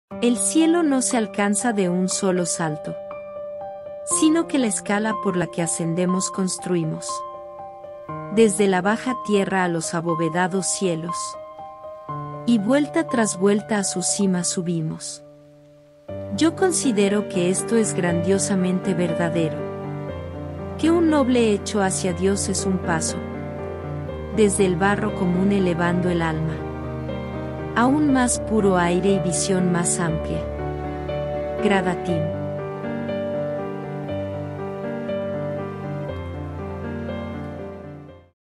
GRADATIM De Josiah Gilbert Holland
El cielo no se alcanza
de un sólo salto;
Sino que la escala por la
que ascendemos construimos
Desde la baja tierra
a los abovedados cielos,
Y vuelta tras vuelta
a sus cimas subimos.

Yo considero que esto es
grandiosamente verdadero:
Que un noble hecho hacia Dios
 es un paso,
Desde el barro común
elevando el alma
A un más puro aire y visión
más amplia.

GRADATIM
De Josiah Gilbert Holland